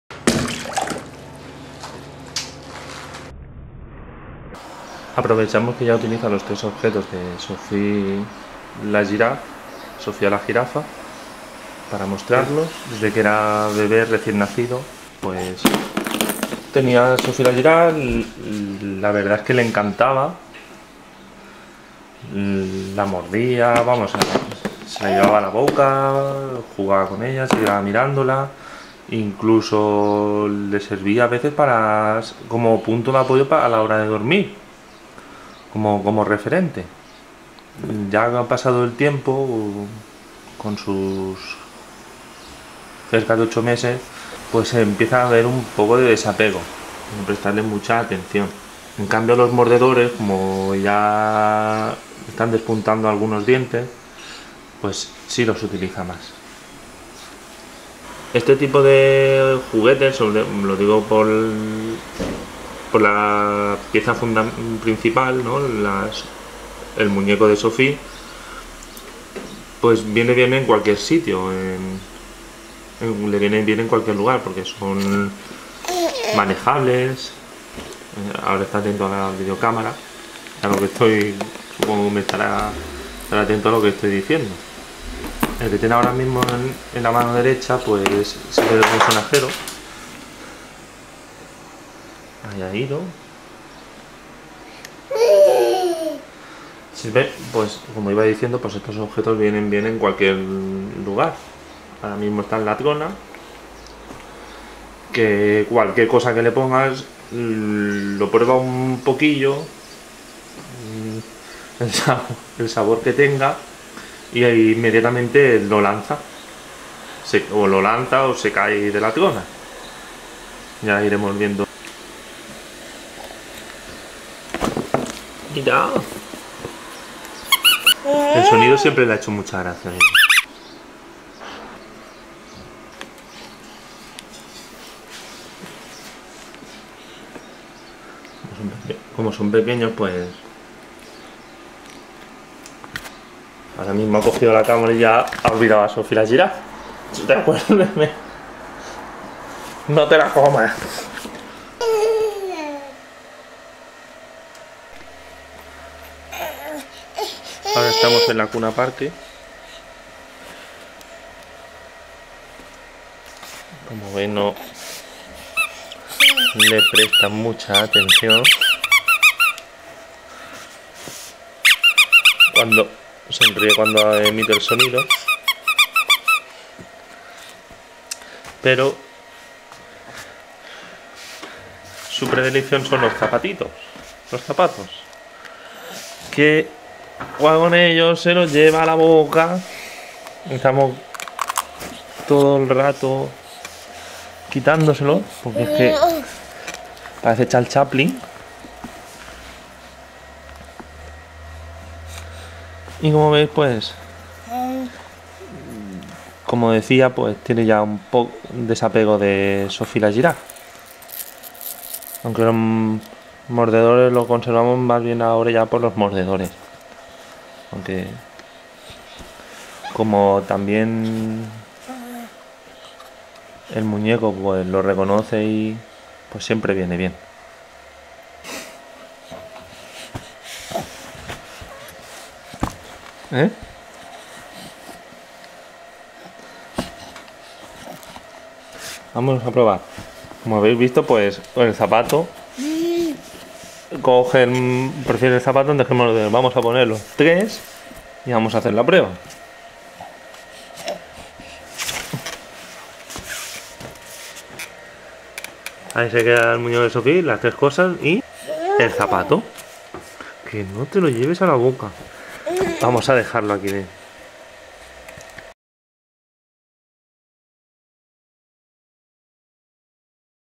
Sophie la girafe (Sofia la jirafa)
Este juguete es un mordedor, 100% orgánico fabricado a partir del caucho, ideal para recién nacidos y cualquier sitio donde vaya (tronas, carritos, parques, alfombras multiactividades...).

En What a Toy! (¡Qué juguete!) lo visionamos pasados unos meses, para comprobar el desapego a él, el uso que hace tanto del muñeco como de los otros mordedores, y el interés por sus zapatos.